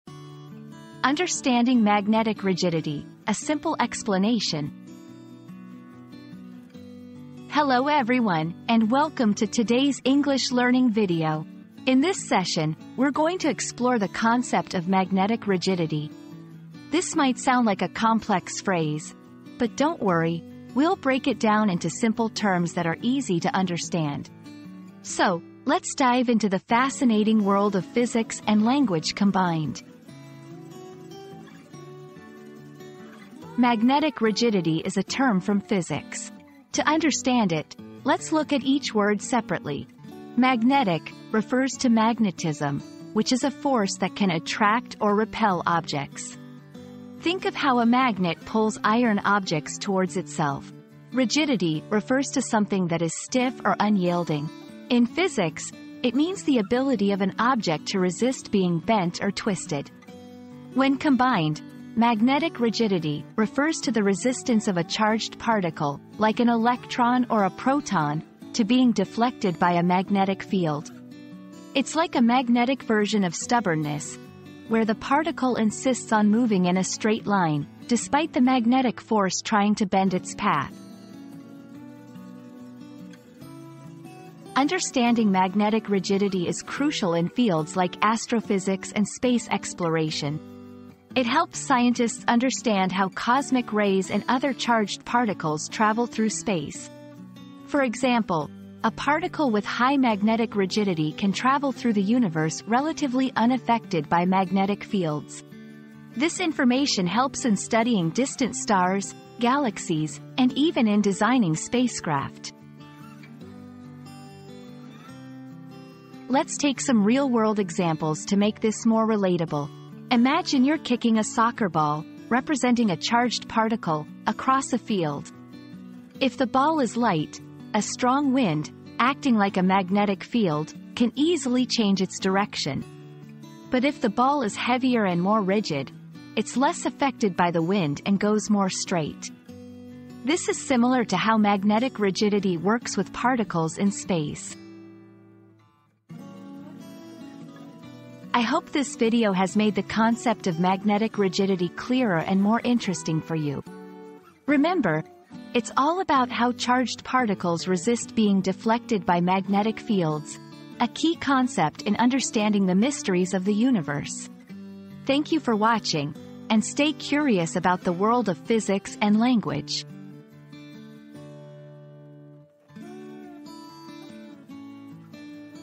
Understanding Magnetic Rigidity: A Simple Explanation
Unlocking Magnetic Rigidity: An Easy Guide • Discover the secrets of magnetic rigidity in this straightforward and informative video. Learn how it works and why it's important in just a few minutes!

00:00 • Introduction - Understanding Magnetic Rigidity: A Simple Explanation
00:33 • What Does "Magnetic Rigidity" Mean?
01:24 • Why is Magnetic Rigidity Important?
01:55 • Real-world Examples